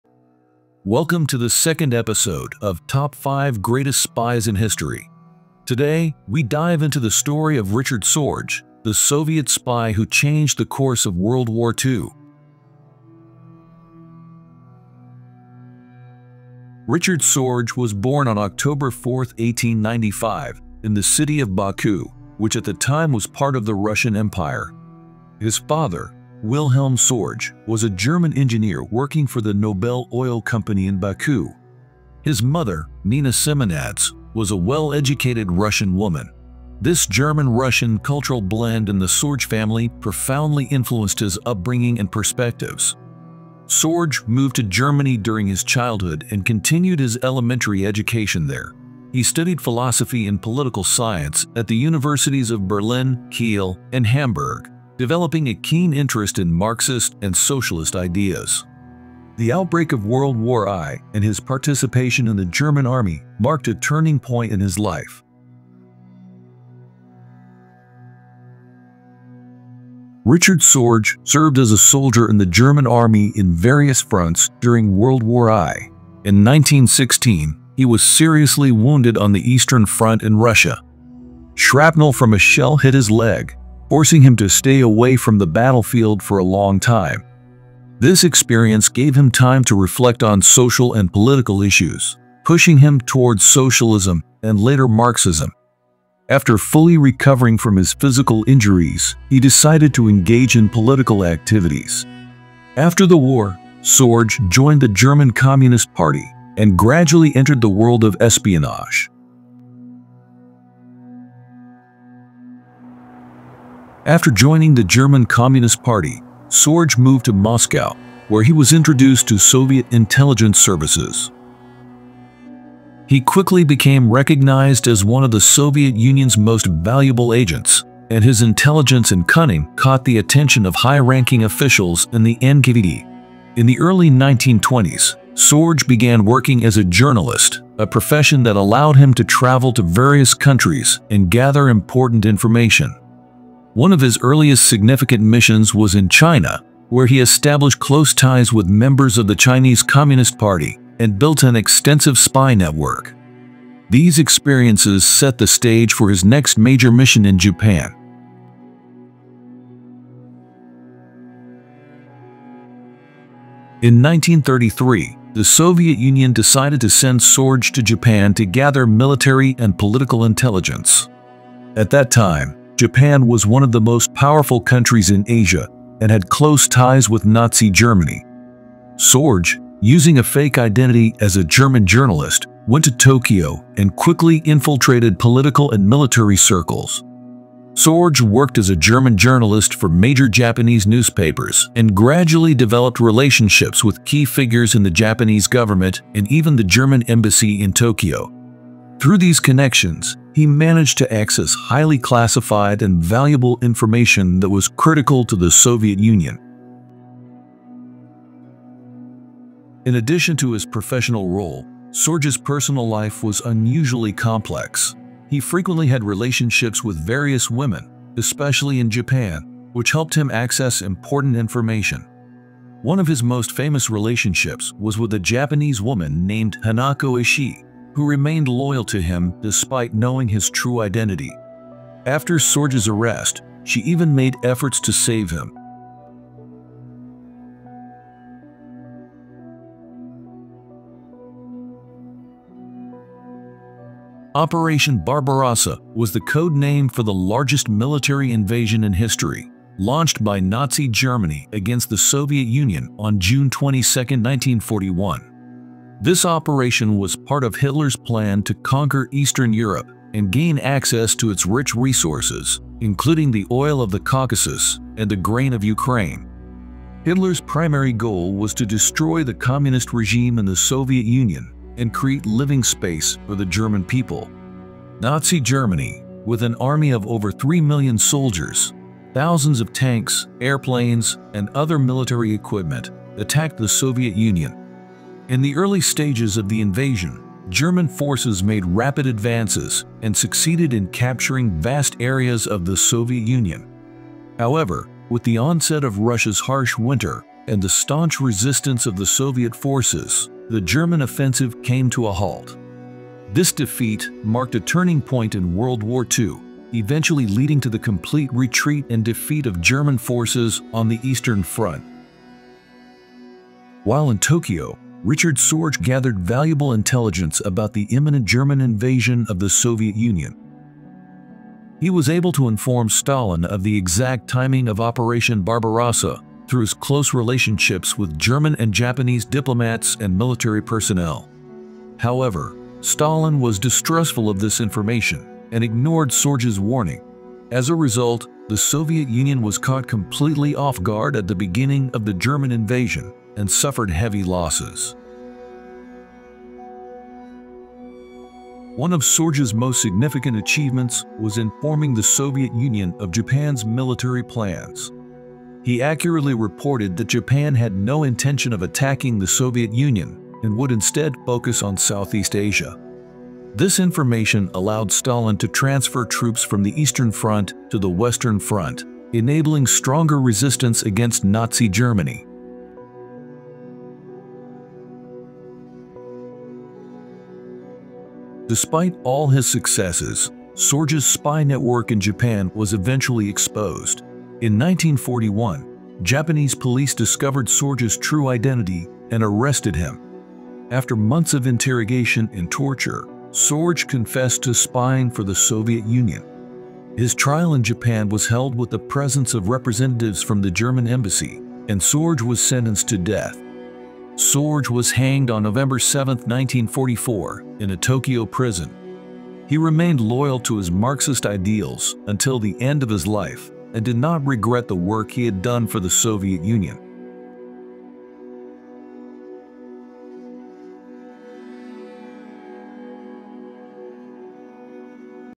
Top 5 Greatest Spies in History: Part 2 The Secrets of Richard Sorge
In the second episode of our Top 5 Greatest Spies in History series, we uncover the story of Richard Sorge, the Soviet spy whose intelligence shaped the outcome of World War II. From his complex network in Japan to his daring double life, Sorge’s tale is one of loyalty, betrayal, and intrigue. Join us as we dive into the secrets, challenges, and risks that made Sorge one of the most impactful spies of his time.